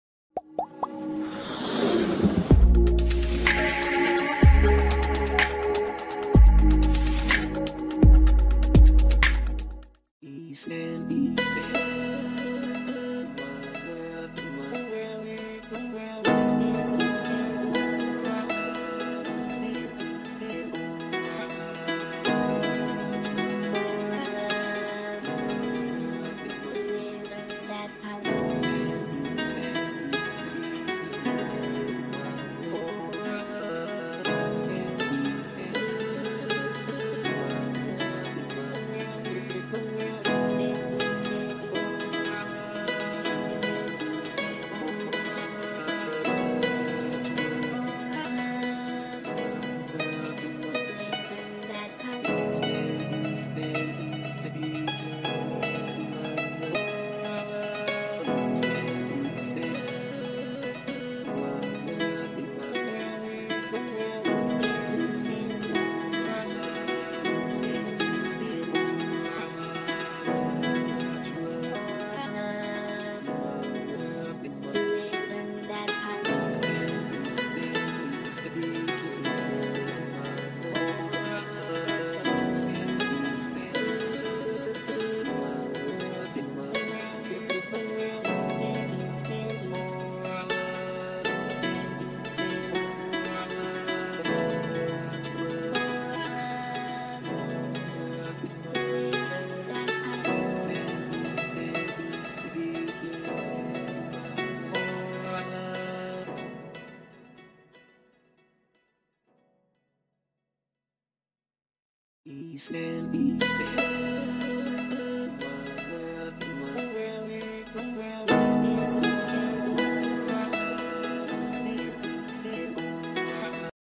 Doc-Nelle tue mani 2, Giusy Buscemi rivela: "La storia di Lucia Ferrari è ispirata a uno psicologo"
Doc-Nelle tue mani 2, Giusy Buscemi rivela: "La storia di Lucia Ferrari è ispirata a uno psicologo" Questa sera andrà in onda una nuova puntata di